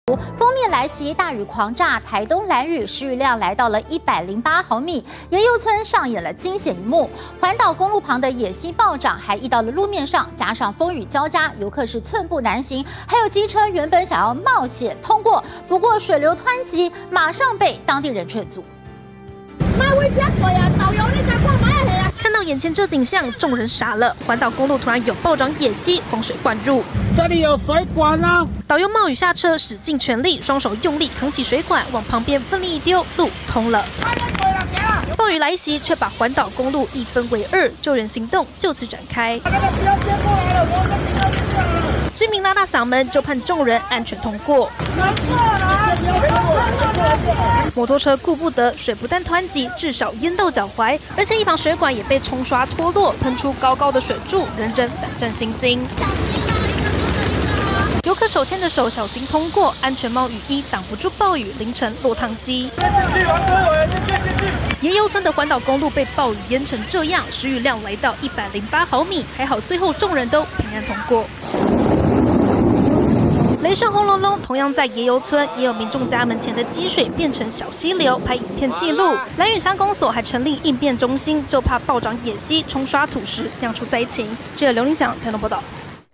時雨量108毫米「路變河」水湍急 雨衣難擋暴雨
鋒面來襲，大雨狂炸台東蘭嶼，時雨量來到108毫米，椰油村上演驚險一幕。環島公路旁的野溪暴漲，還溢到路面上，加上風與交加，遊客寸步難行，還有機車原本想要冒險騎車通過，不過水流湍急，馬上被當地人勸阻。